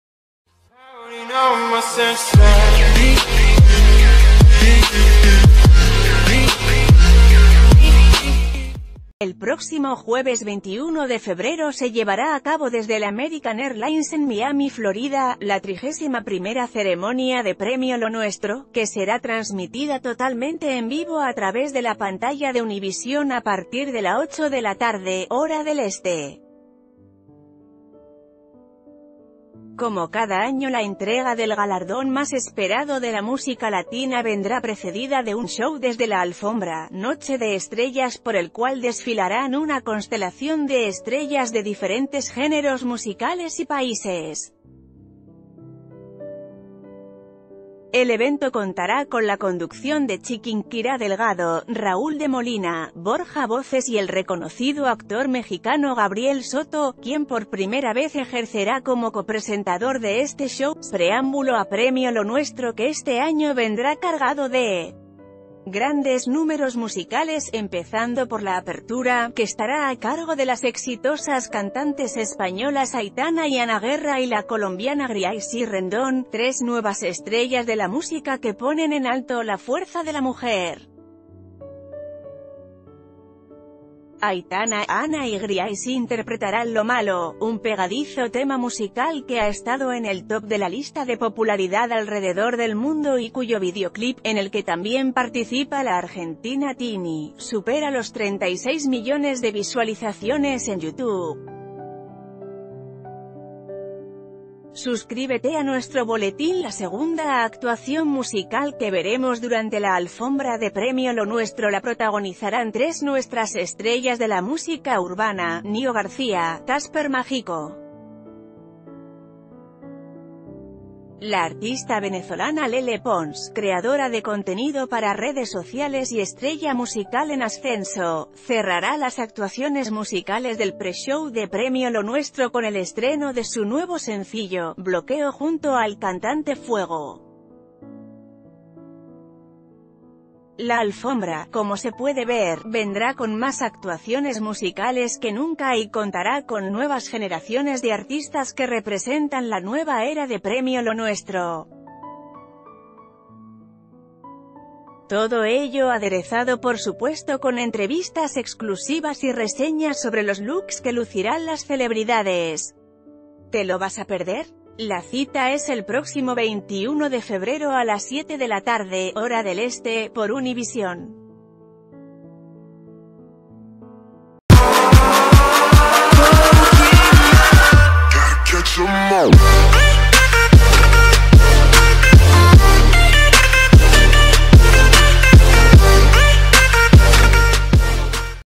Gabriel Soto entre los presentadores del pre-show de Premio Lo Nuestro: te contamos todas las nov...
Gabriel Soto entre los presentadores del pre-show de Premio Lo Nuestro: te contamos todas las novedades
El próximo jueves 21 de febrero se llevará a cabo desde el American Airlines en Miami, Florida, la 31ª ceremonia de Premio Lo nuestro, que será transmitida totalmente en vivo a través de la pantalla de Univision a partir de la 8 p.m., hora del Este. Como cada año la entrega del galardón más esperado...